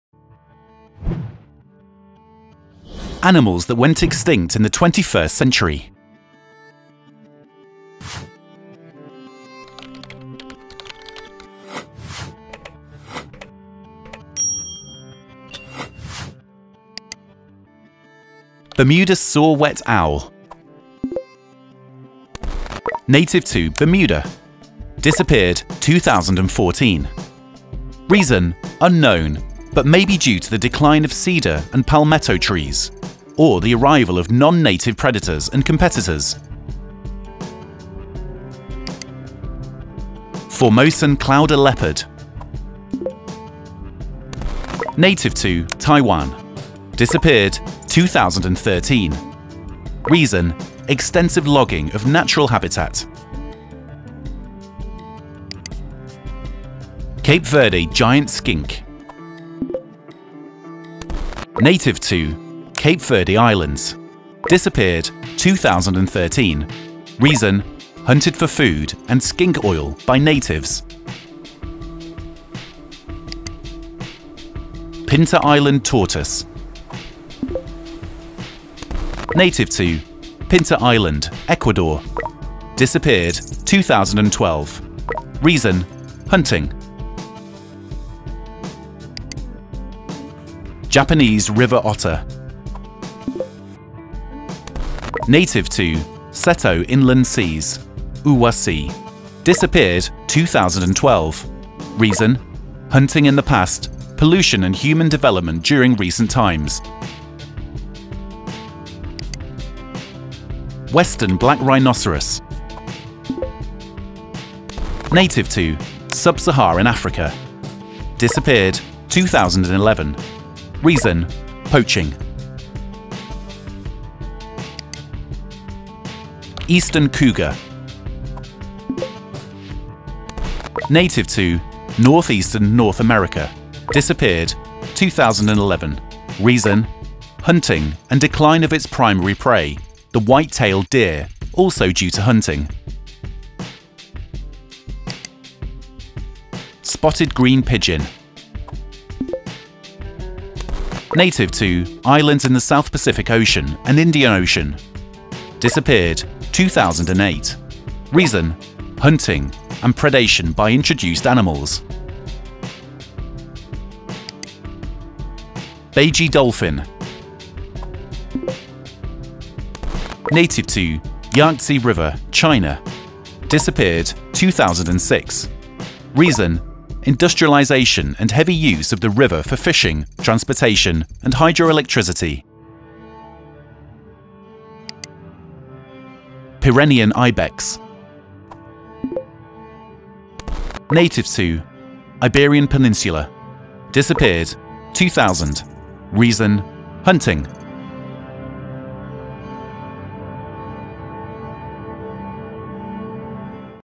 Animals That Went Extinct In The 21st Century
According to new research, biodiversity across much of Earth has dropped below safe levels. Here are some of the species we've lost this century—catalyzed by overhunting, logging, and manmade climate change.